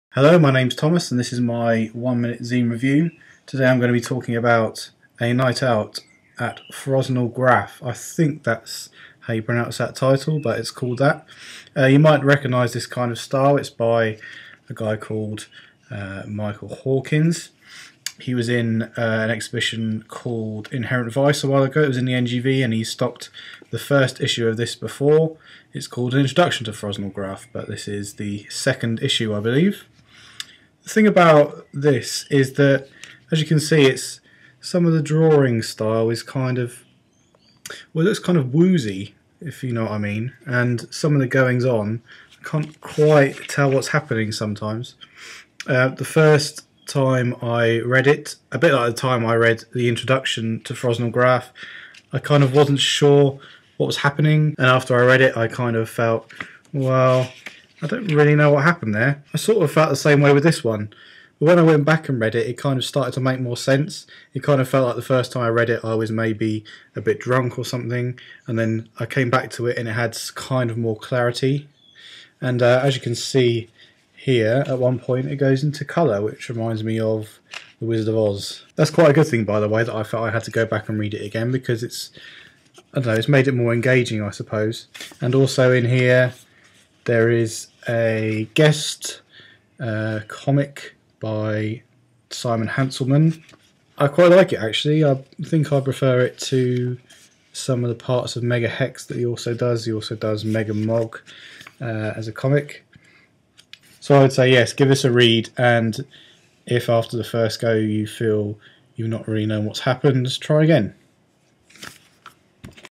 Sticky Minute #1 - A Night Out In Frosnall Graaf review
Here's the first episode of Sticky Minute, where a zine gets reviewed in under two minutes (we're not that strict). Here the new comic 'A Night In Frosnall Graaf' by Michael Hawkins gets the once over.

You can look at more of Mr. Hawkins' work here: artbyhawkins.blogspot.com.au

and you can buy this comic from Sticky's mail order department